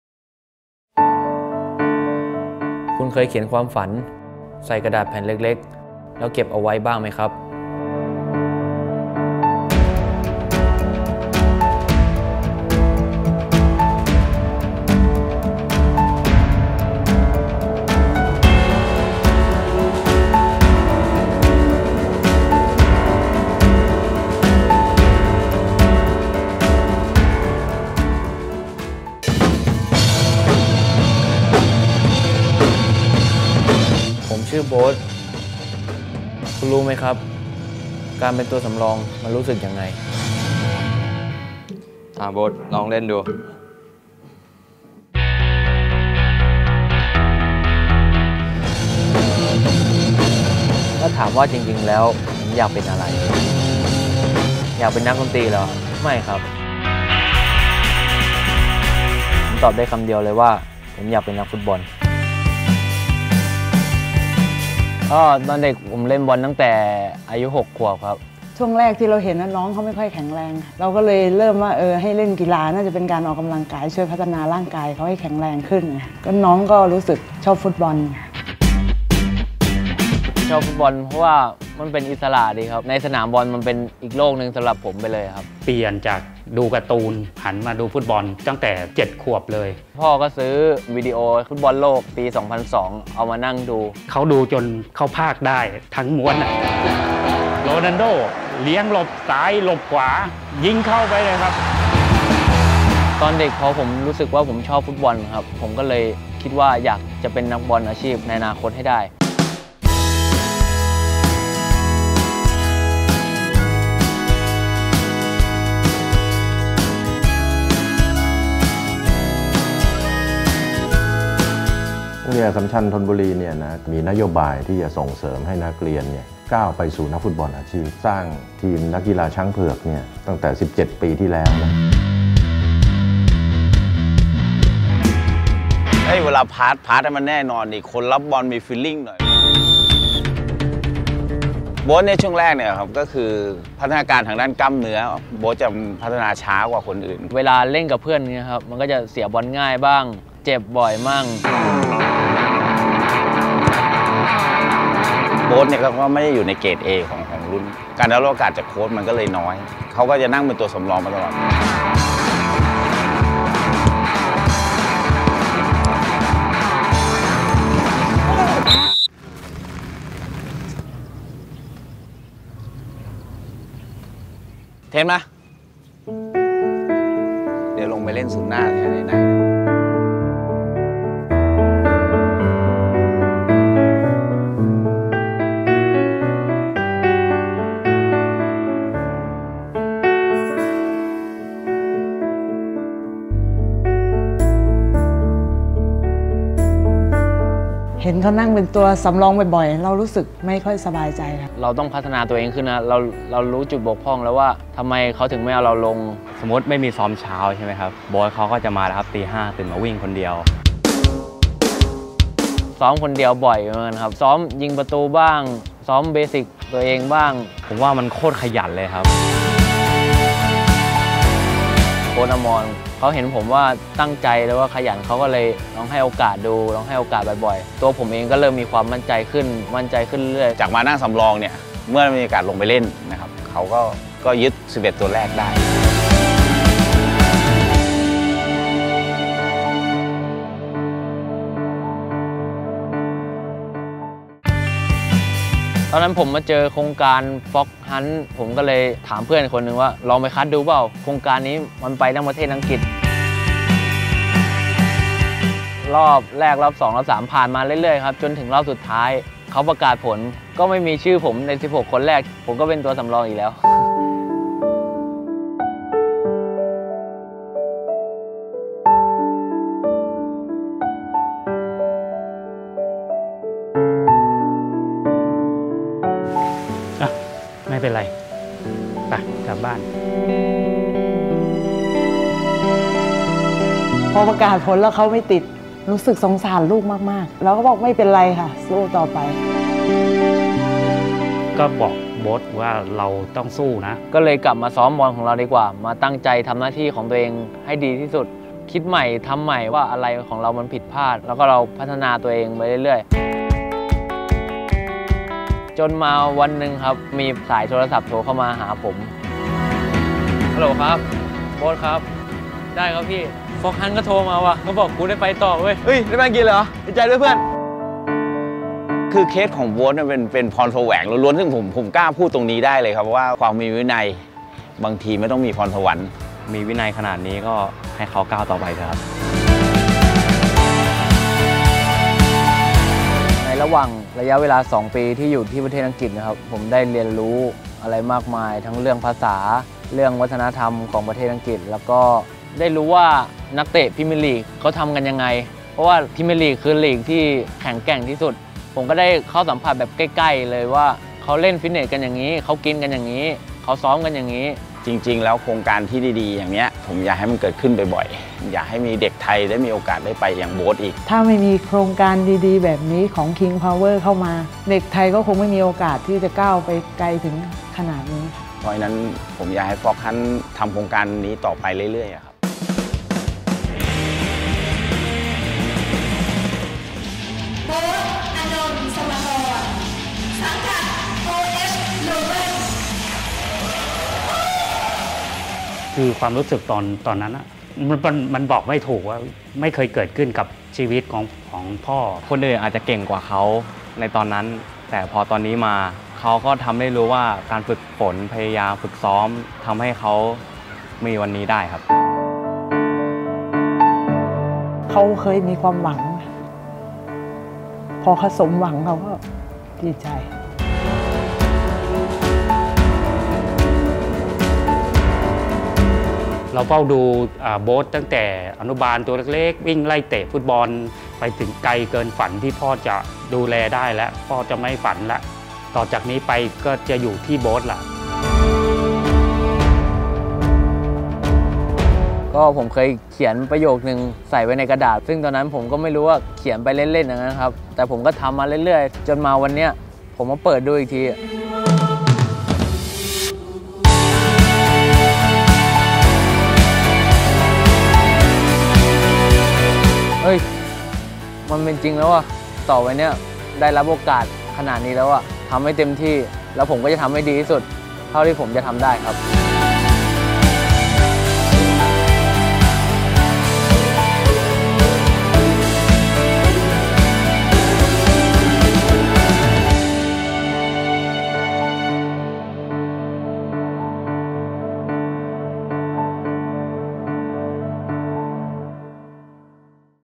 "โบ๊ต อนนต์ สมากร" ตัวสำรอง (ทั้งปี)
"โบ๊ต อนนต์ สมากร" เด็กน้อยจากสมุทรสาคร ผู้ที่ใฝ่ฝันอยากจะเป็นนักเตะอาชีพ แต่กลับได้แต่นั่งเป็นตัวสำรอง จากจุดนี้เองไม่ได้ทำให้ โบ๊ต ท้อแท้แต่อย่างใด ในทางกลับกัน มันได้กลายเป็นแรงผลักดันให้ โบ๊ต ได้พัฒนาตัวเองขึ้นเรื่อยๆ จนได้เซ็นสัญญาอาชีพกับทีม โอเอช ลูเวิน ในที่สุด